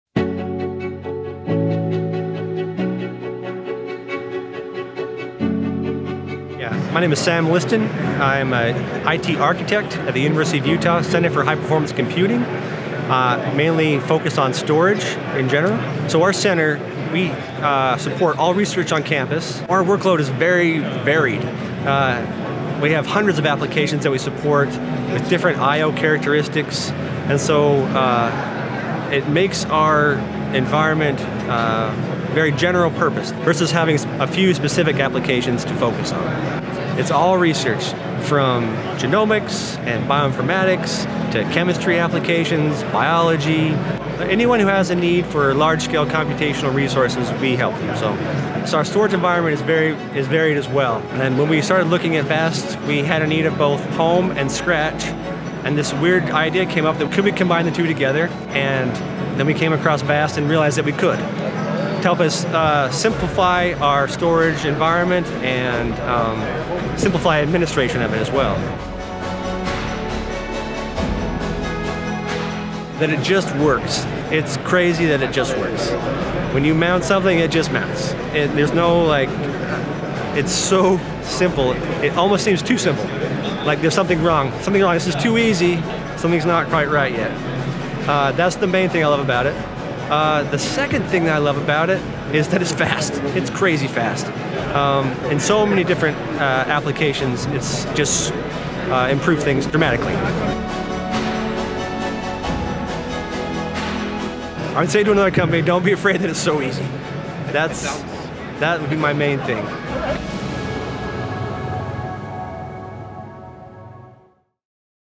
The VAST Experience: University of Utah HPC
VAST Data’s Universal Storage is a data storage platform that combines high-performance storage with low-cost, scalable capacity. The platform is designed to support a wide range of workloads and use cases, including genomics, bioinfomatics, chemistry applications, and large scale computational resources.

In this video Sam Liston, Senior IT Architect at The University of Utah Center for HPC talks about supporting hundreds of varied research applications with VAST to simplify storage environments and administration.